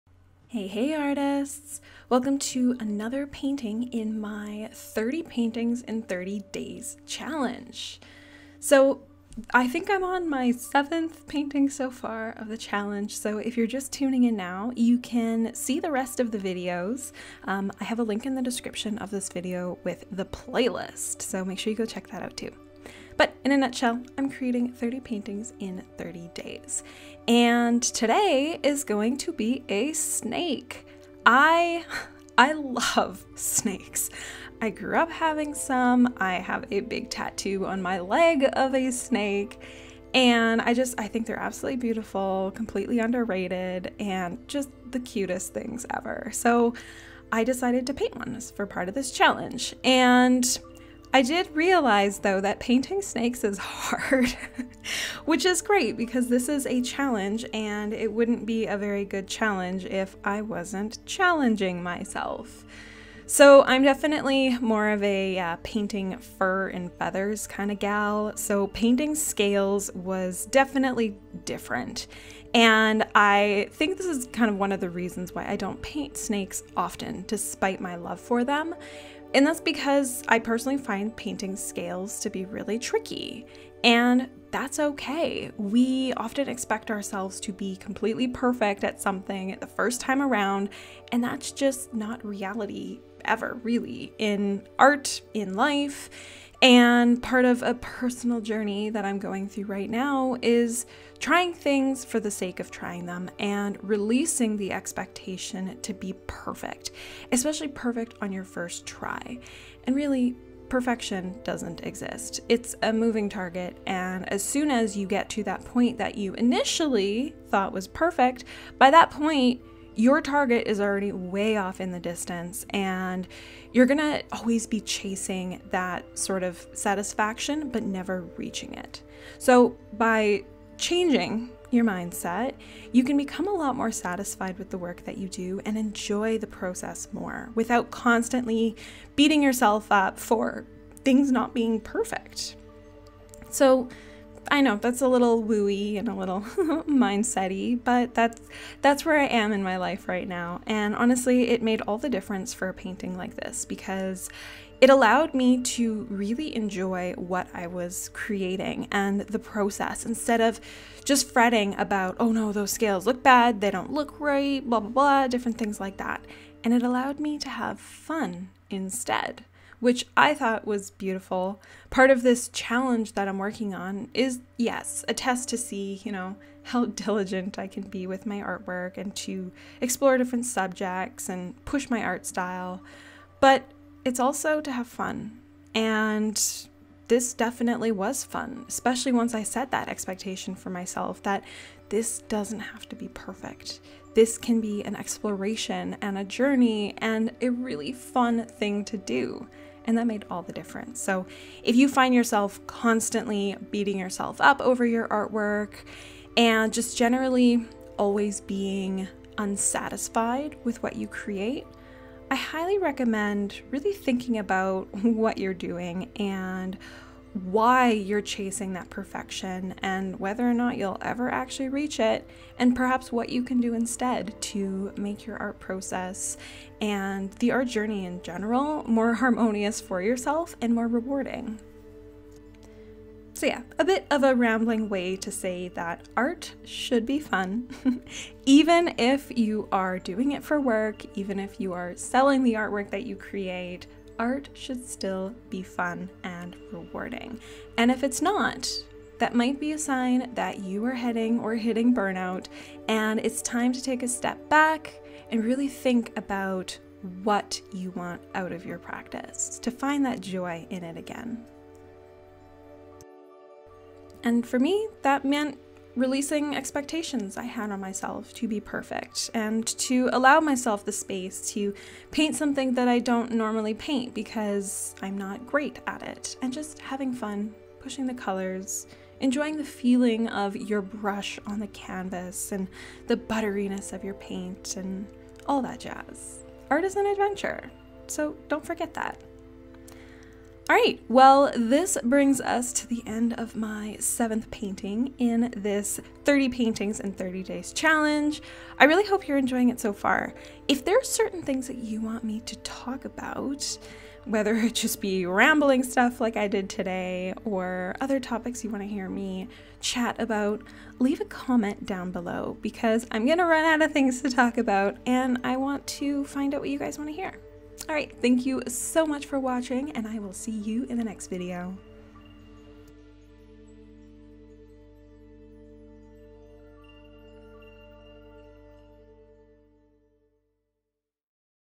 Should Art Be FUN?? | Snake OIL PAINTING | 30 Paintings in 30 Days Challenge
Hey friends!

Join me while I start to dig into this brand new challenge that I have granted myself - one that will likely have me kicking and screaming by the end of it.

The 30 Paintings in 30 Days challenge!

Each day I will be uploading the timelapse footage complete with me rambling about something art-related (or maybe even nothing to do with art).

► Want to paint realistic wildlife easily?  ◄
The Wildlife Painting Academy is your secret weapon for painting beautiful animals.

Materials Used:

Paint:
Winsor & Newton Winton Oil Colour Paint
Gamblin Artist's Oil Colors

Mediums and other liquids:
Winsor & Newton Liquin Original
Mona Lisa Linseed Oil
Mona Lisa Odorless Paint Thinner
Gamblin Gamvar
Mona Lisa Gold Leaf Size Adhesive
Gold Leaf (Imitation)
Gold Leaf (Genuine)
Liquitex White Gesso

Other:
H.J. White Taklon brushes

Filming Equipment:
Canon Rebel T5i